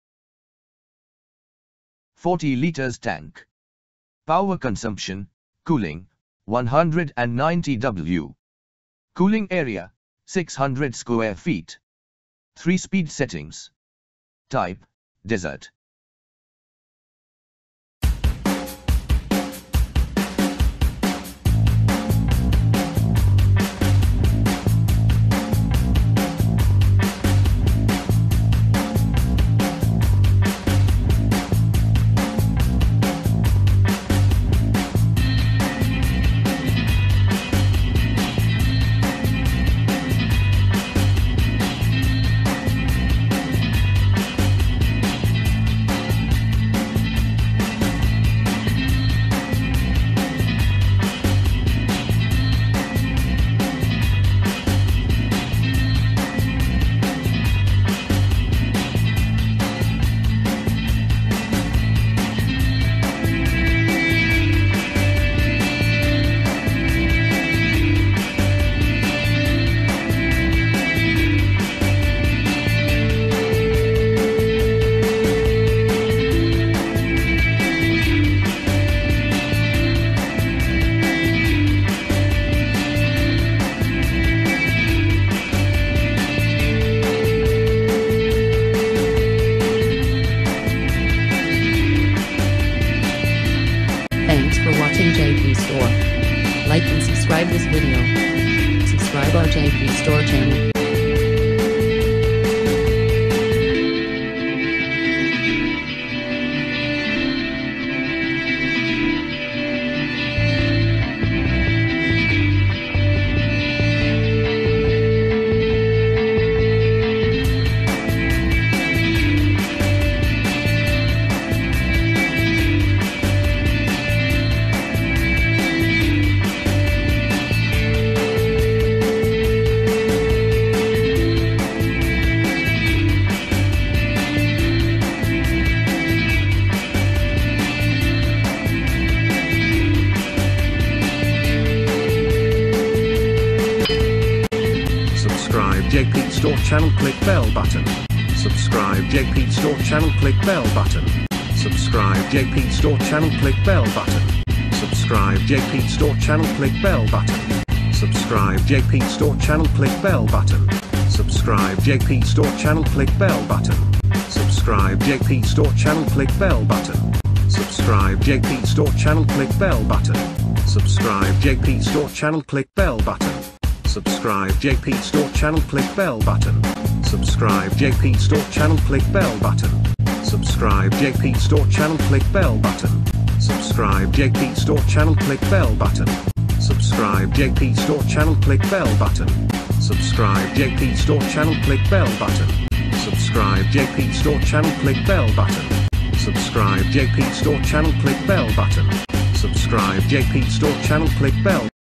Kenstar TURBO COOL SUPER WITH REMOTE Desert Air Cooler (White, 40 Litres)
40 Litres Tank
Power Consumption - Cooling: 190 W
Cooling Area: 600 sq ft
3 Speed Settings
Type: Desert